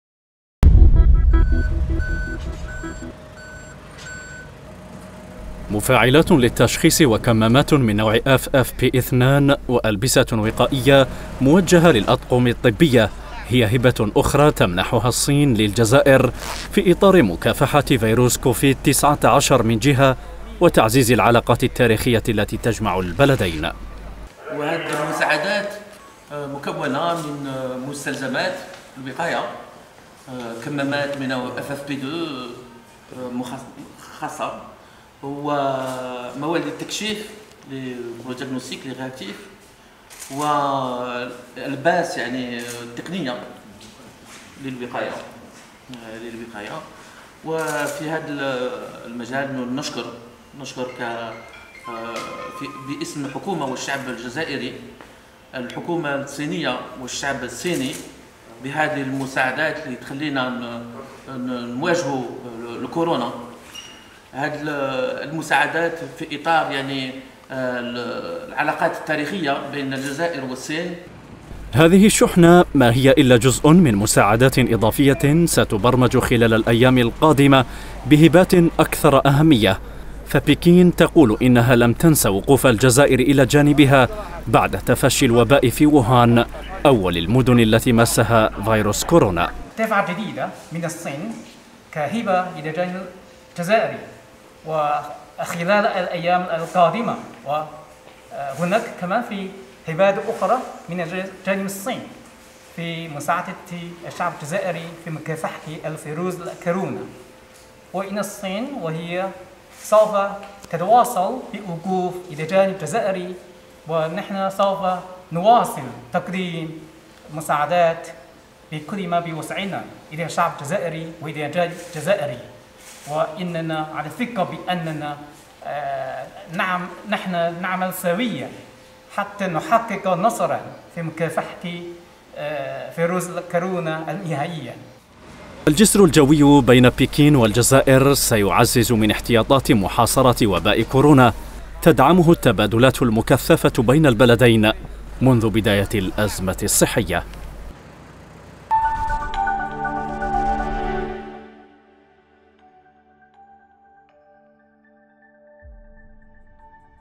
الجزائر تتلقى هبة طبية أخرى من الصين
الجزائر (وأج)- تلقت الجزائر في إطار مكافحة فيروس كورونا المستجد هبة أخرى من الحكومة الصينية متكونة أساسا من كمامات جراحية وألبسة وقائية طبية.